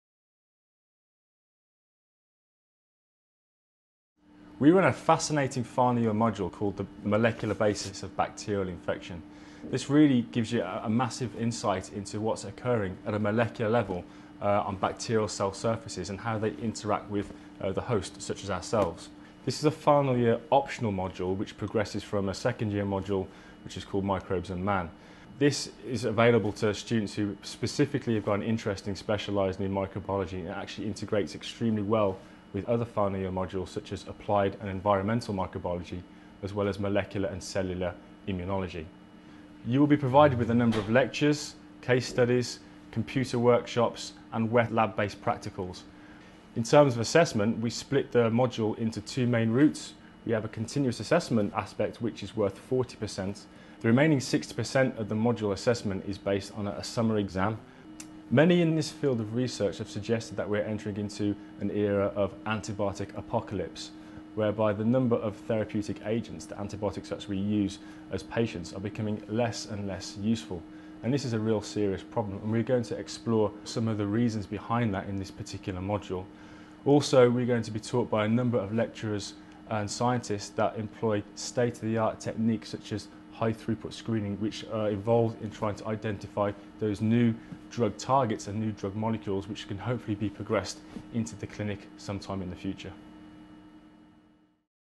BIO305 Molecular basis of bacterial infection - Module - School of Biosciences
Dr Luke Alderwick, Lecturer and Director of the Birmingham Drug Discovery Facility in the School of Biosciences at the University of Birmingham, describes the optional third year module Molecular basis of bacterial infection.